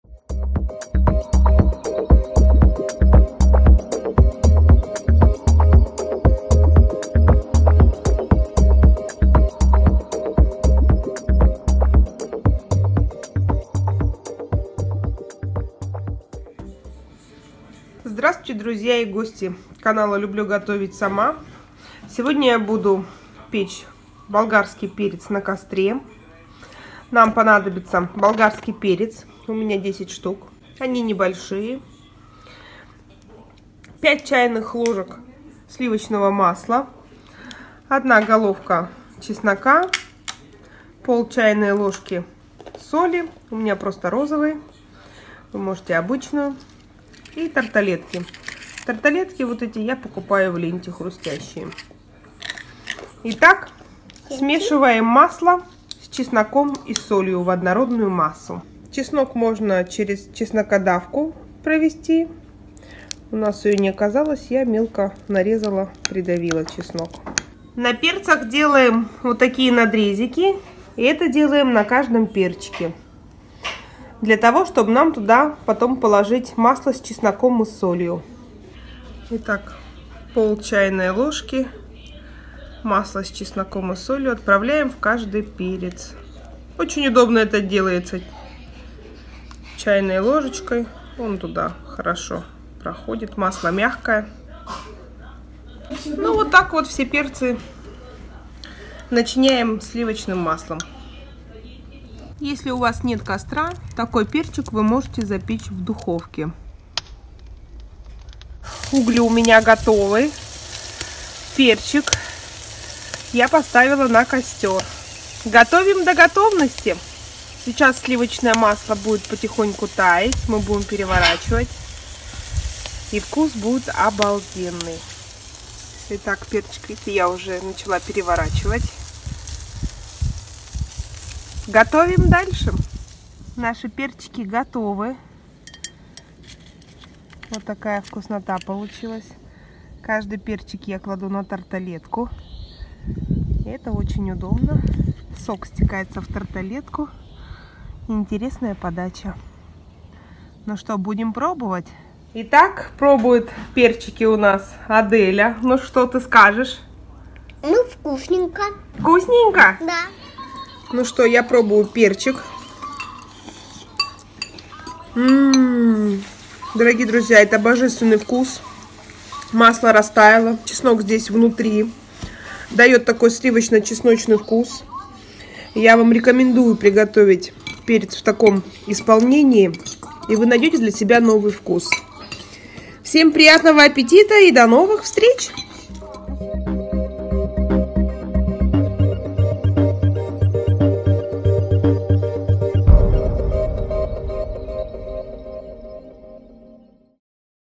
Перчик на углях ! Обалденный 👌
НАМ ПОНАДОБИТСЯ:
БОЛГАРСКИЙ ПЕРЕЦ 10 ШТ.
СЛИВОЧНОЕ МАСЛО 5 Ч.Л.
ТАРТАЛЕТКИ 10 ШТ.
  RECIPE:
 Sweet Pepper 10 ,
 butter 5st ,
 garlic 1 🧄 ,
 salt 0,5 st ,
 tartlets 10.